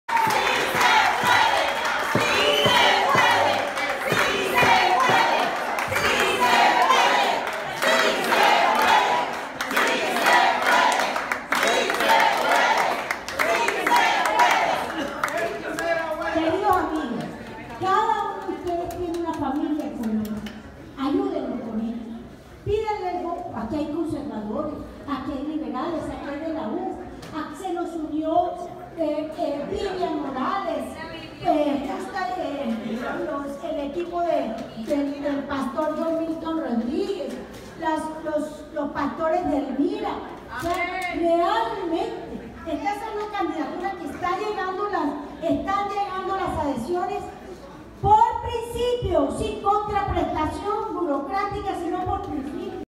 Centro Democratico NY por Duque
El grupo religioso MIRA se une al Centro Democrático para apoyar al candidato Iván Duque en las elecciones a la presidencia de Colombia 2018.